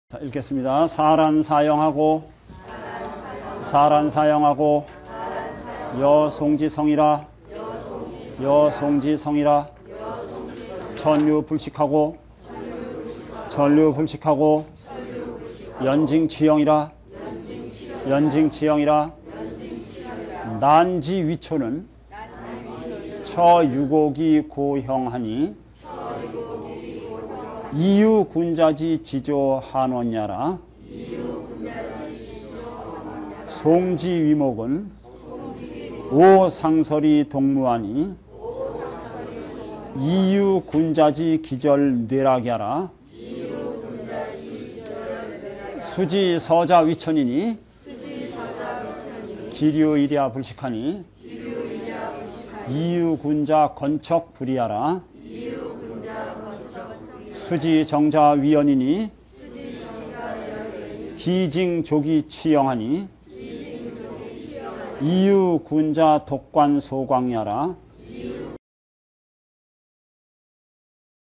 034 似 蘭 斯 馨하고 如 松 之 盛이라 - 035 川 流 不 息하고 淵 澄 取 映이라
蘭之爲艸(草)는 處幽谷而孤馨하니 以喩君子之志操閒遠也라 松之爲木은 傲霜雪而獨茂하니 以喩君子之氣節磊落也라

水之逝者爲川이니 其流日夜不息하니 以喩君子乾惕不已也라 水之停者爲淵이니 其澄以取映하니 以喩君子獨觀昭曠也라